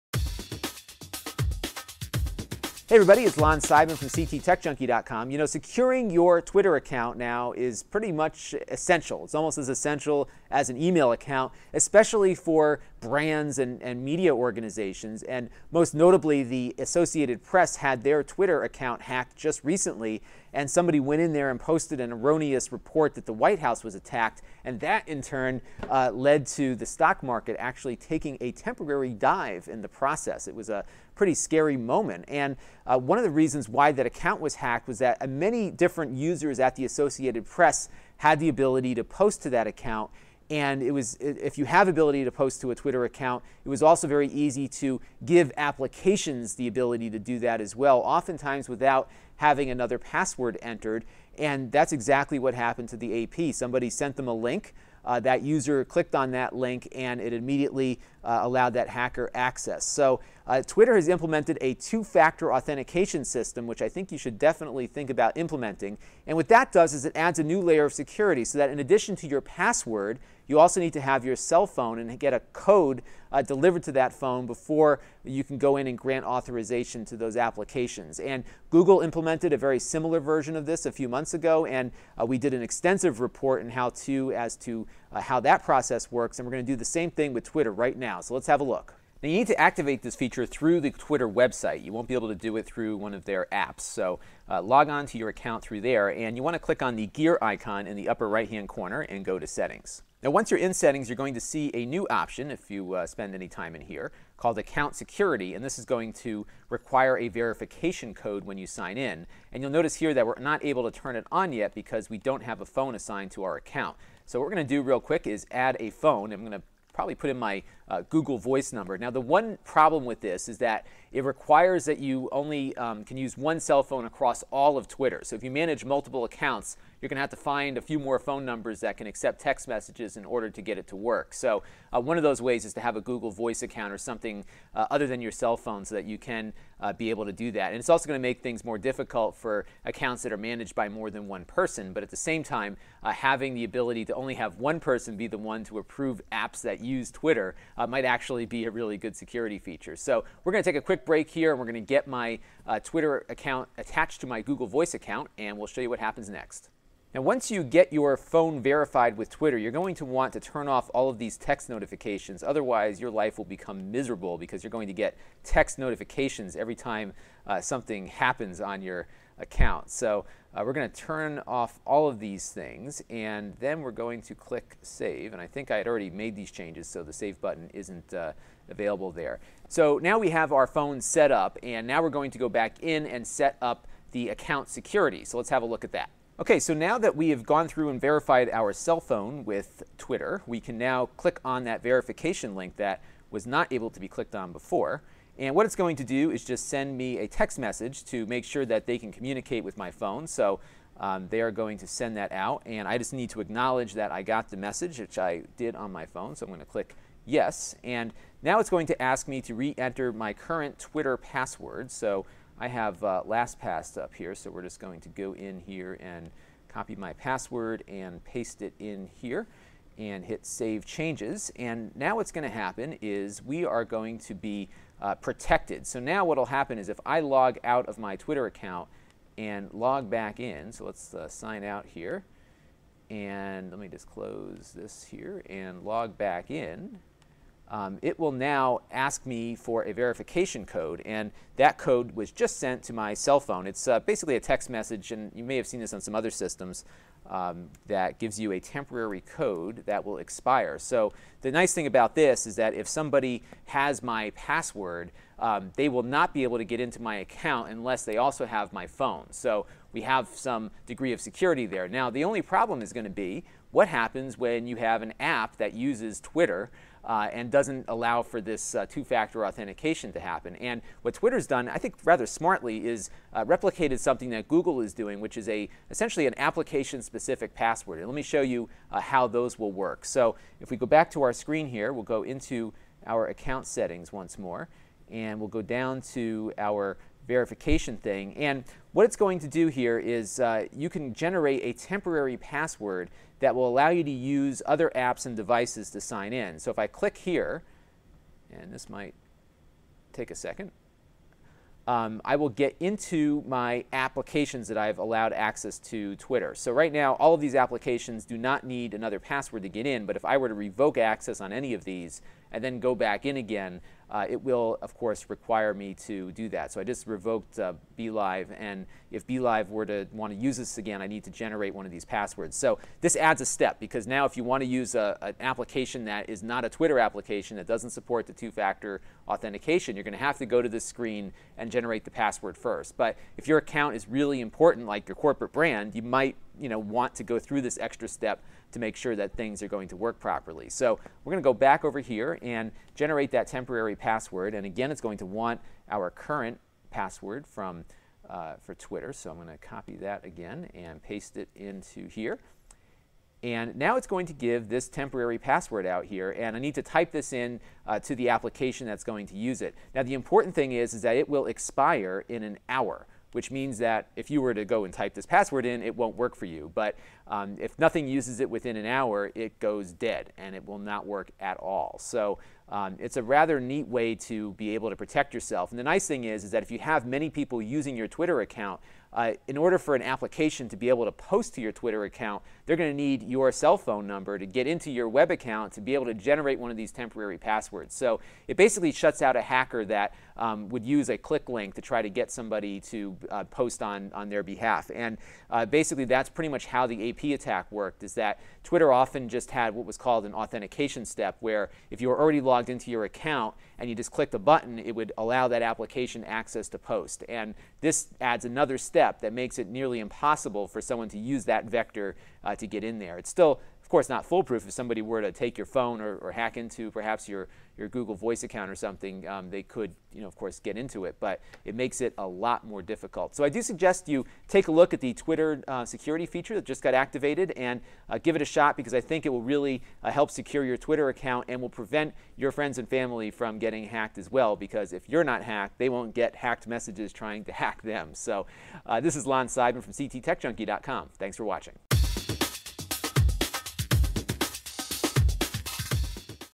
How to Setup Twitter Two Factor Authentication for Greater Security
Twitter recently implemented two factor (two step) authentication to make it more difficult for hackers to compromise accounts.  The change came after the Associated Press twitter account was hacked. The process involves using a cell phone that is sent a verification number via a text message.